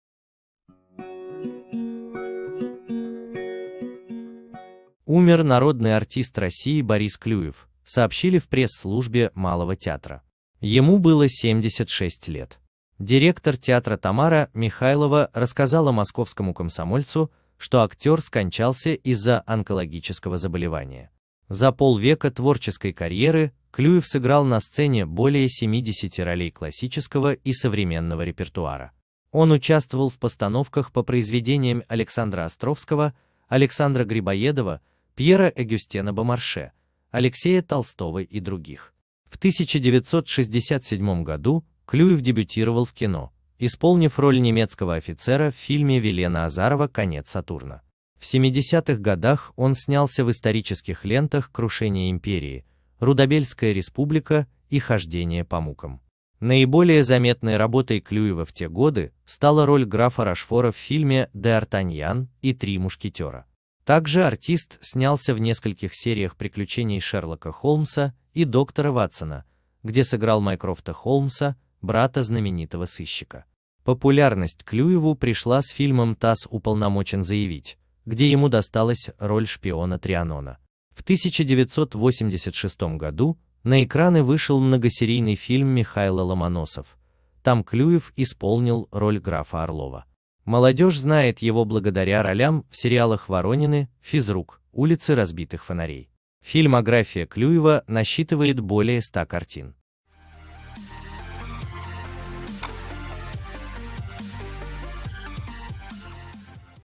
Умер звезда сериала «Воронины» Борис Клюев
Вечером 1 сентября стало известно, что народный России Борис Клюев умер в возрасте 76 лет.

Причиной смерти стала тяжелая болезнь, как заявила директор Малого театра Тамара Михайлова. Конечно, в стороне не остались и другие коллеги актера.